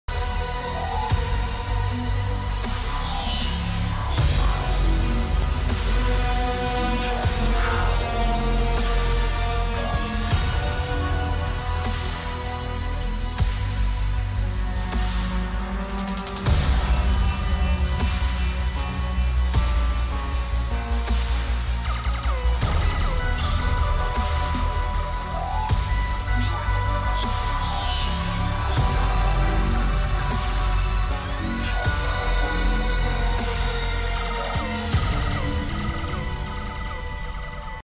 👀
this skip can be useful in races at some times if you could time it right